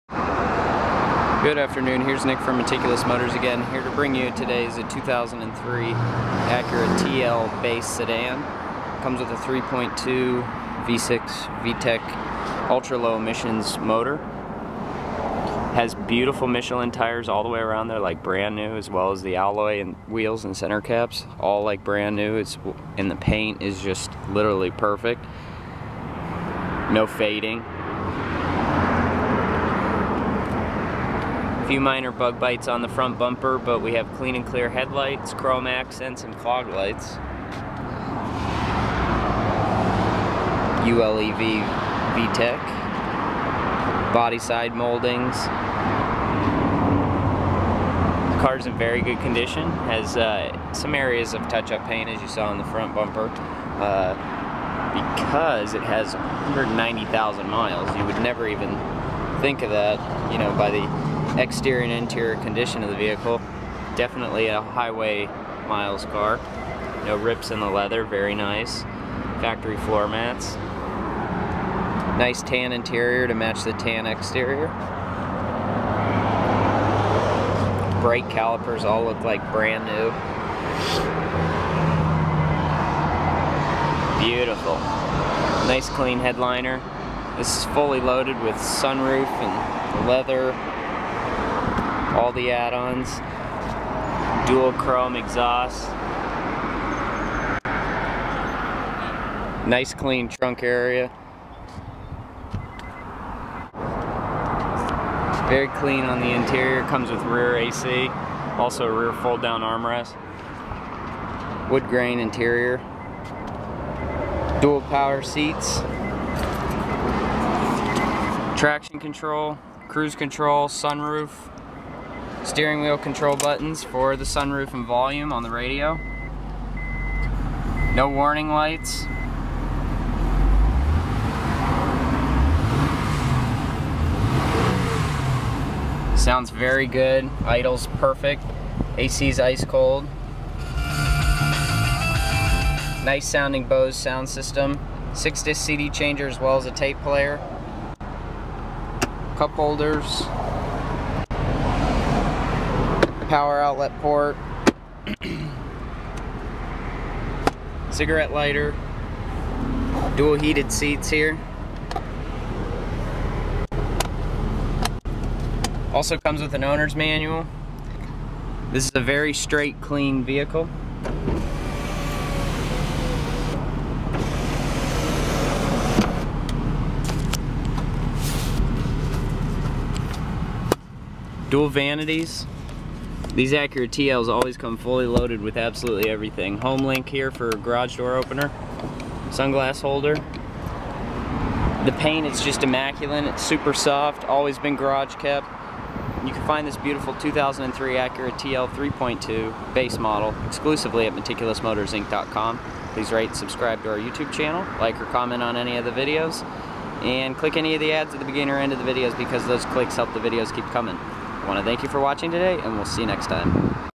SOLD 2003 Acura TL 3.2 Base Meticulous Motors Inc Florida For Sale
2003 Acura TL 3.2. Fully loaded with all of the options such as: power windows/seats/mirrors/locks, heated seats, heated mirrors, Bose surround sound, auto dimming rear view mirror, sunroof, cruise control, leather, 5 speed automatic transmission, sequential sport shift, and TCS system. This vehicle is in awesome shape for the miles. Come by and see this gorgeous Acura TL today!